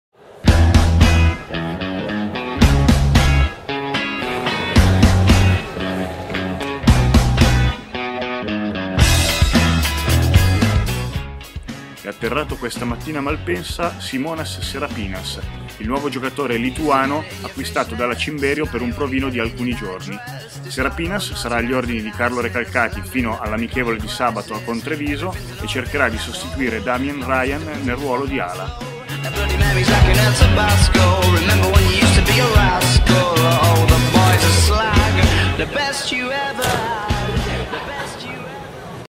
L'arrivo di Simonas Serapinas
È arrivato a Malpensa il lituano Simonas Serapinas. Sosterrà un provino alla Cimberio.
Ha ventinove anni il prossimo 24 febbraio, ala di 1,98 di altezza, Serapinas ha militato in squadre di primo livello come lo Zalgiris Kaunas e l'Aris Salonicco e vanta esperienze importanti in Polonia e Ucraina con il Prokom e il Mariupol. Rallentato da intoppi burocratici e qualche problema fisico, in questa stagione non ha giocato, pur passando in Italia per un provino - non superato - a Forlì, in LegaDue.